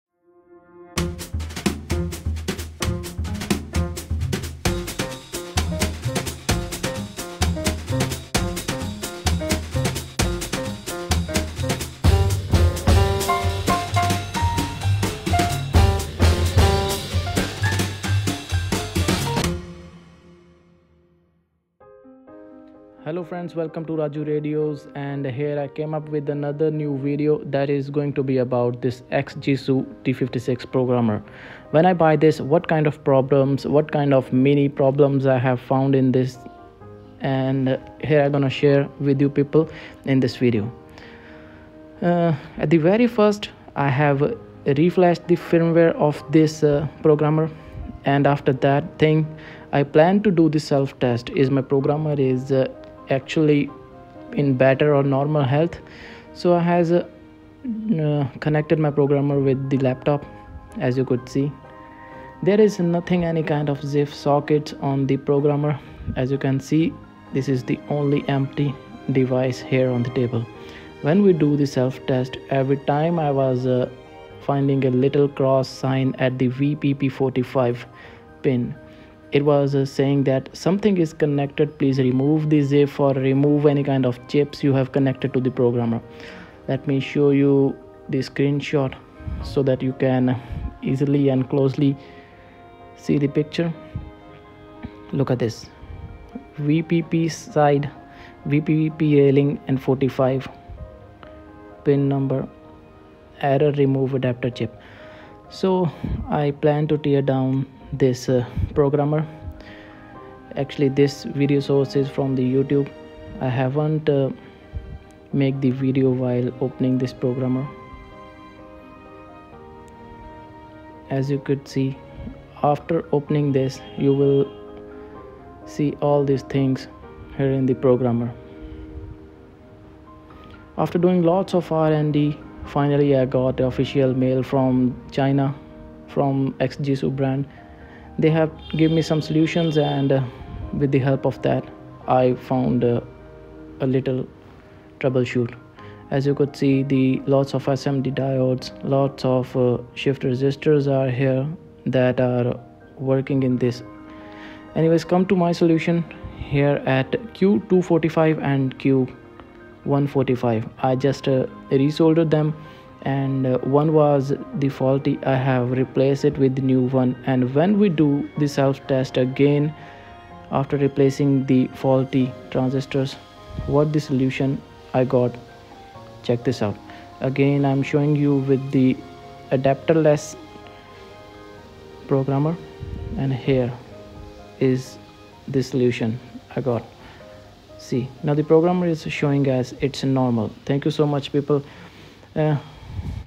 XGECU T56 SYSTEM SELF-CHECK PROBLEM. ERROR! REMOVE ADAPTER/CHIP. TEARDOWN VIDEO
here in this video i done the troubleshoot for myself with a brand new device for me which i have recently  introduced in my stuff as a tool. so this little guy got a error in factory manufacturing time, even it was very little that it may be affecting any kind of stuff while i ll be working but still, it will be a thing, a little thing in my mind to make it fixed for future requirements. so i have planned to open the product.check out the video.